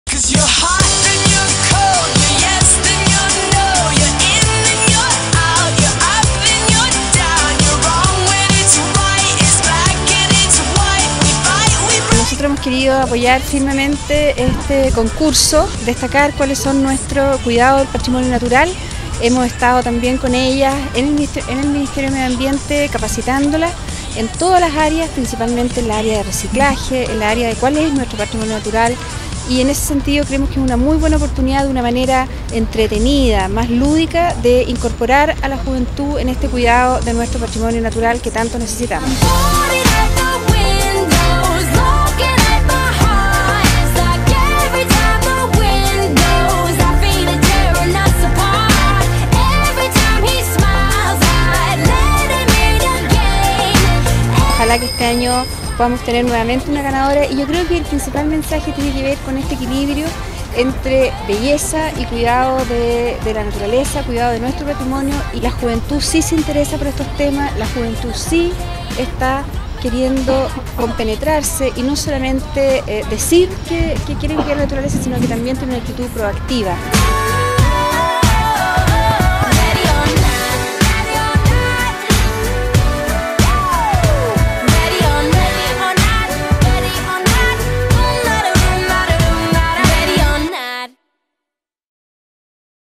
Presentación de candidatas Miss Earth Chile 2013
En el Punto Verde del Parque Araucano se presentaron a las 12 candidatas del concurso Nuestra Belleza Chile, que busca a la representante nacional para el Miss Earth 2013, a realizarse en Filipinas!